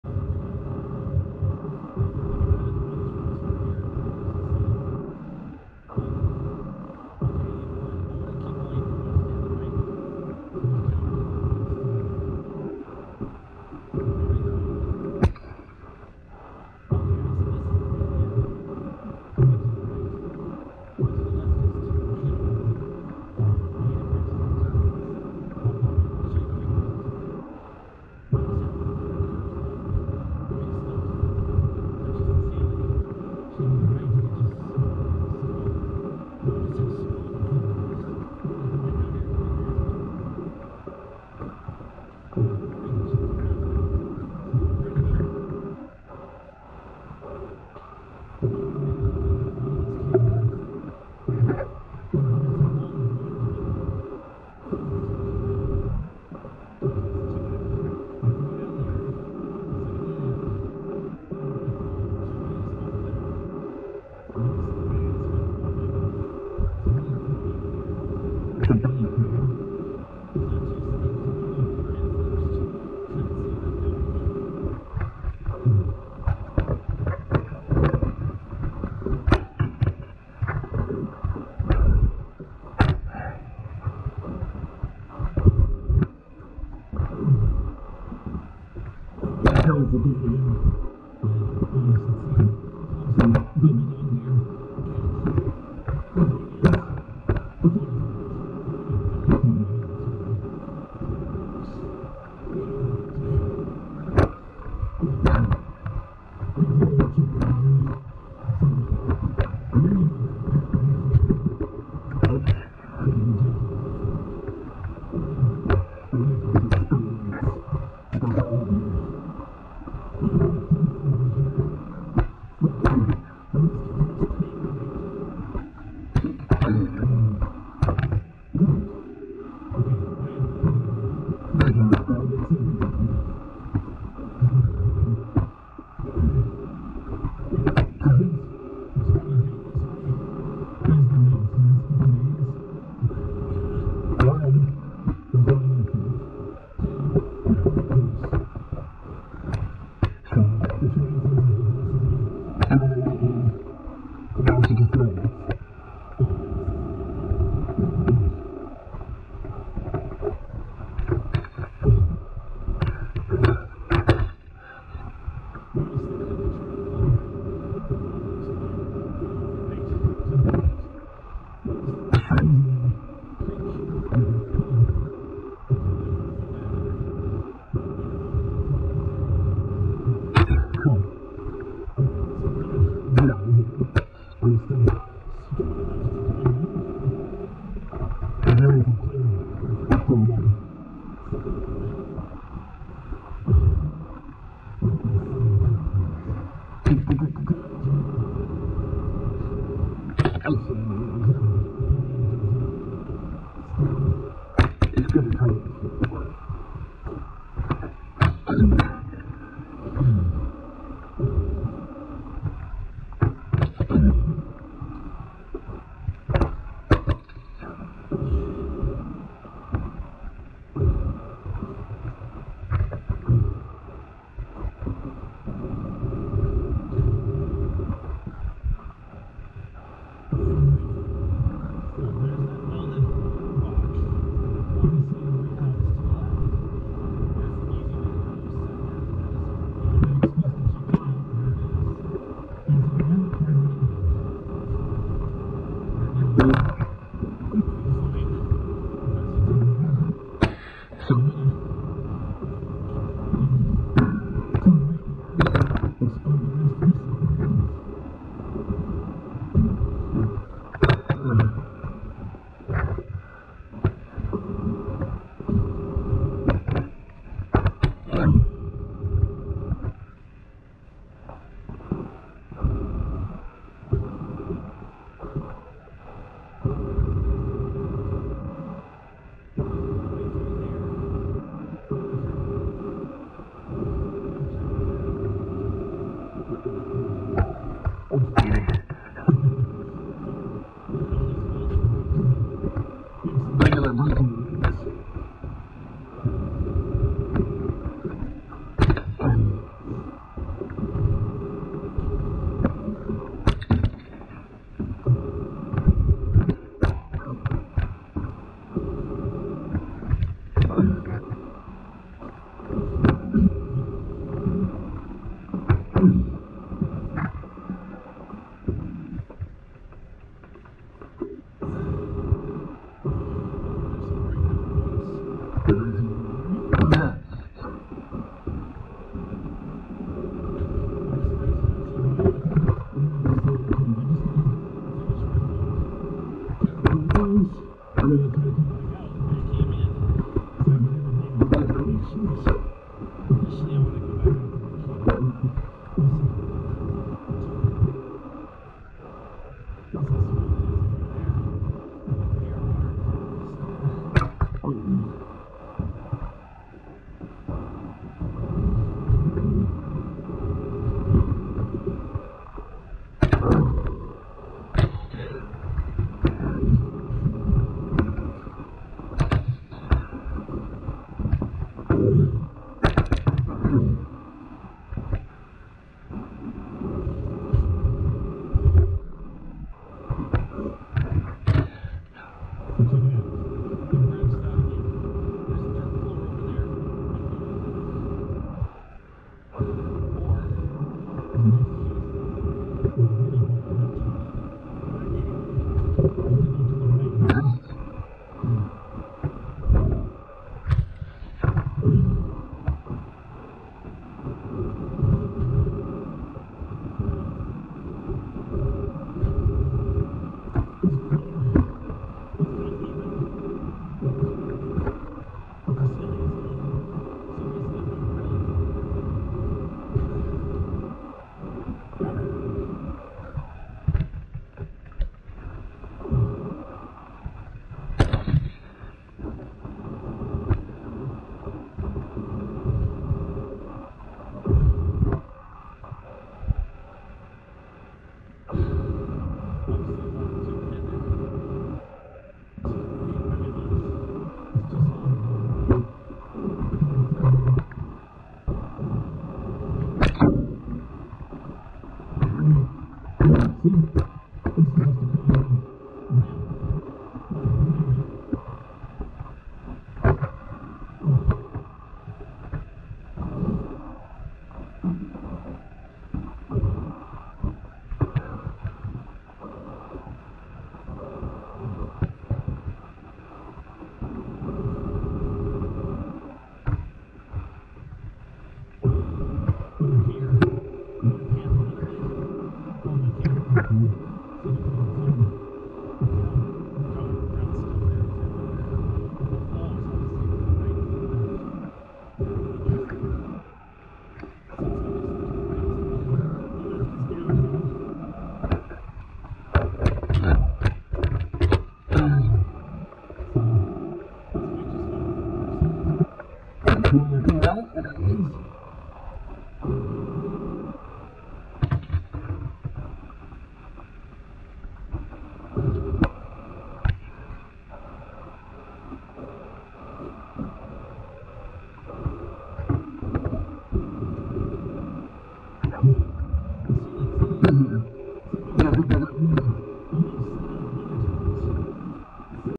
Dead Horse Cave, video 8, part 9, 2160p60
This is Dead Horse Cave in Skamania County, Washington.  Video 8 starts at the lower entrance, goes to the stream passage, goes up the stream passage to the large side passage, then explores the large side passage.